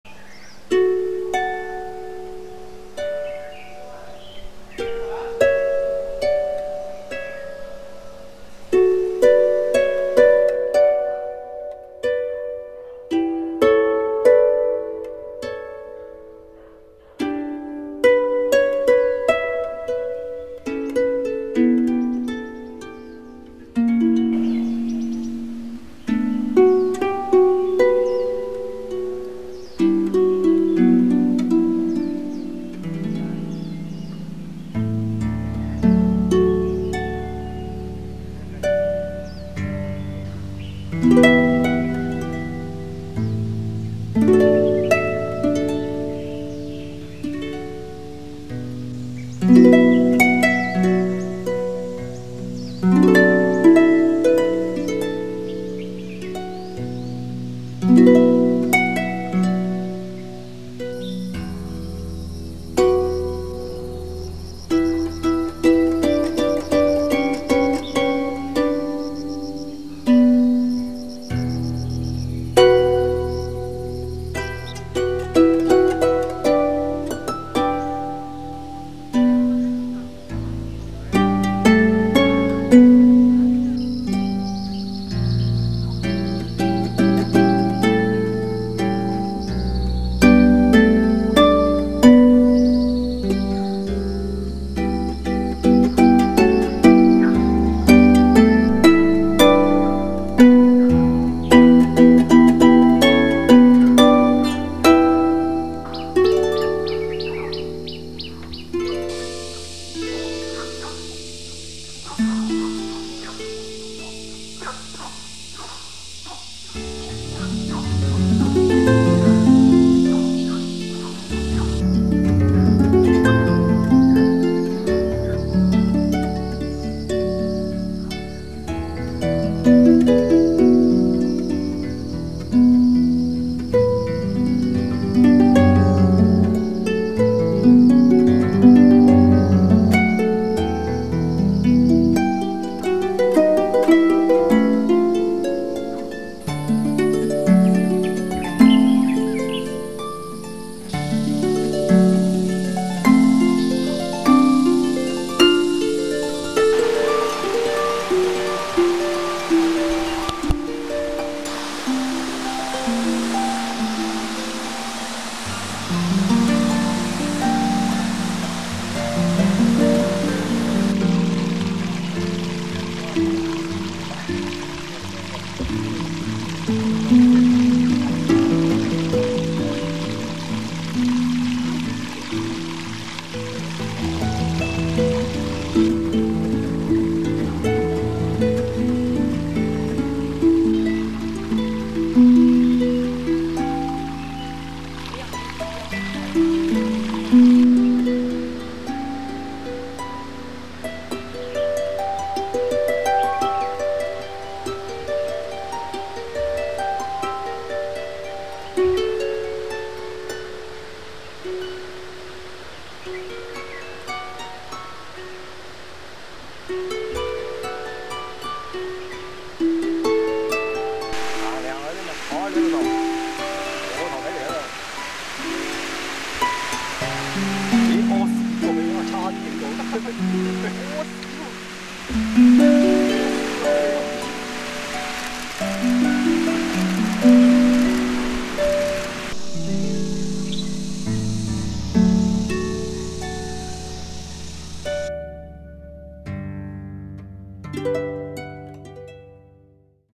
五月香港生態之旅
分別於大埔滘及蠔涌攝錄，使用了Canon EOS 5D Mark II拍攝。